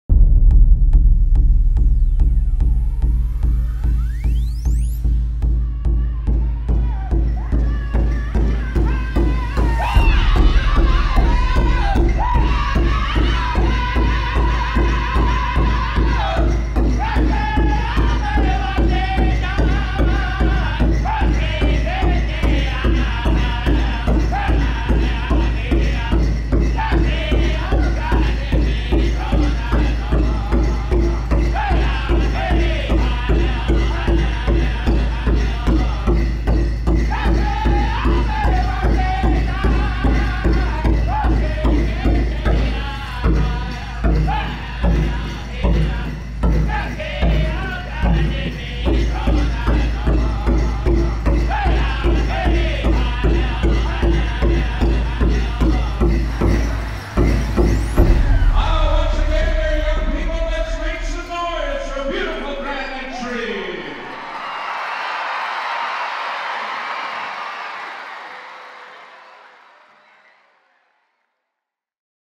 Indigenous Peoples History Month Pow Wow & Dance Celebration 2022
On June 7, 2022, Saskatoon Public Schools hosted the Indigenous People History Month Pow Wow & Dance Celebration. Thousands of students and hundreds of dancers, drummers, and other participants were at SaskTel Centre for the school division's first pow wow gathering in more than two years.